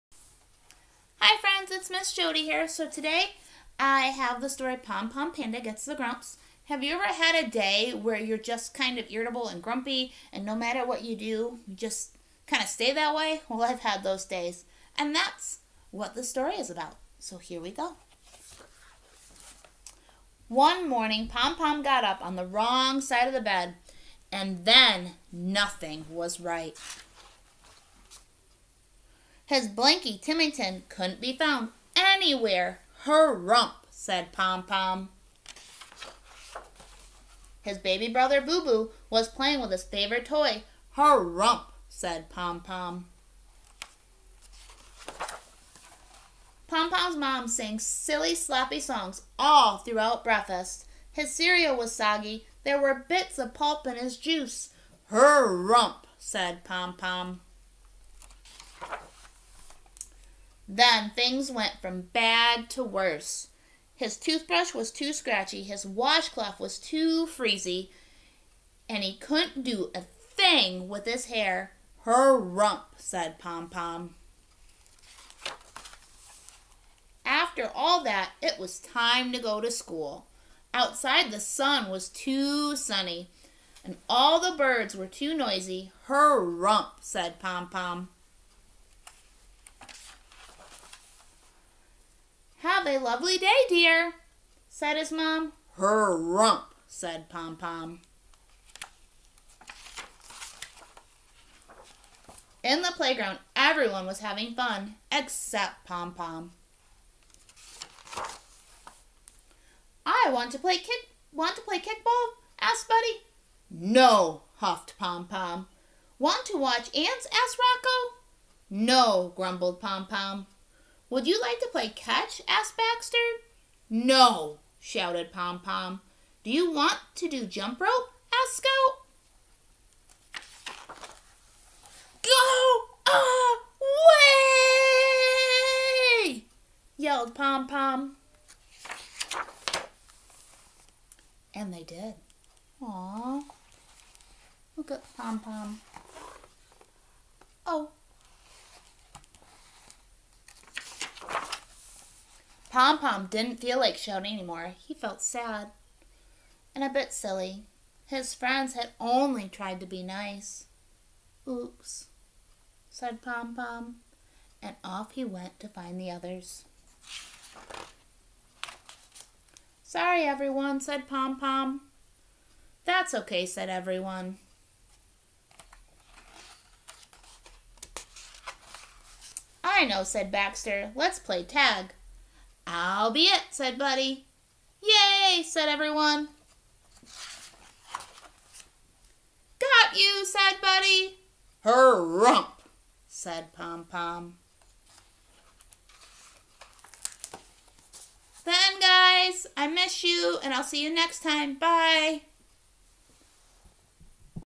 Pom Pom Panda get the grumps
Ms. Jodie reads Pom Pom Panda get the grumps by Sophy Henn